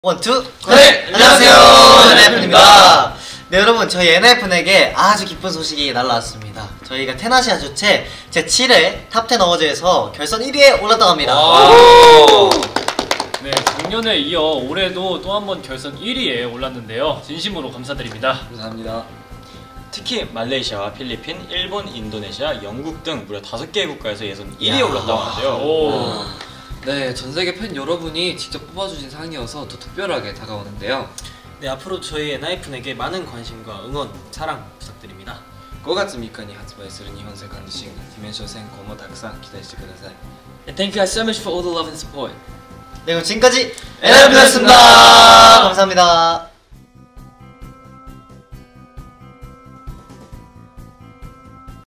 [TTA-글로벌 1위] 엔하이픈(ENHYPEN), '글로벌 라이징 스타' 증명의 연속
그룹 엔하이픈(ENHYPEN)이 제7회 톱텐 어워즈(TOP TEN AWARDS, 이하 TTA)'에서 글로벌 1위를 차지하며 '4세대 핫 아이콘'임을 또 증명했다.

텐아시아가 주관하는 'TTA'는 한국, 미국, 영국, 멕시코, 중국, 일본, 태국, 인도네시아, 말레이시아, 필리핀 등 10개 지역 K팝 팬들이 온라인·모바일 투표로 최고의 아티스트를 가리는 행사다.

엔하이픈은 지난 2월까지 치러진 예선에서 필리핀, 일본, 인도네시아, 영국, 말레이시아 5개국 1위를 차지했다. 글로벌 팬의 지지를 얻은 K팝 스타 10개팀과 대결 끝에 엔하이픈이 63.23%(163만 5646표)라는 압도적 득표율로 최종 1위를 차지했다. 특히 엔하이픈의 'TTA' 글로벌 1위는 지난 6회에 이어 2년 연속이다